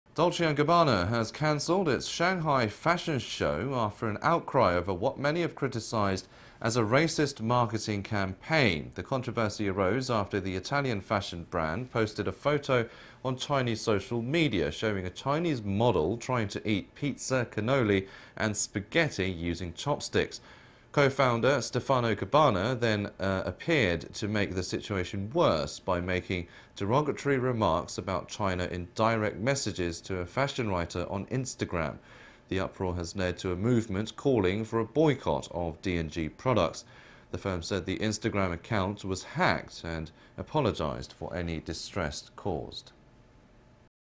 Dolce & Gabbana cancels Shanghai fashion show amid racism row
"중국 모욕했다"…돌체앤가바나, 스타들 보이콧에 中패션쇼 취소

Dolce & Gabbana has cancelled its Shanghai fashion show after an outcry over what many have criticized as a racist marketing campaign.
The controversy arose after the Italian fashion brand posted a photo on Chinese social media showing a Chinese model trying to eat pizza, cannoli and spaghetti using chopsticks.
Co-founder Stefano Gabbana then appeared to make the situation worse by making derogatory remarks about China in direct messages to a fashion writer on Instagram.
The uproar has led to a movement calling for a boycott of D&G products.
The firm said the Instagram account was hacked and apologized for any distress caused.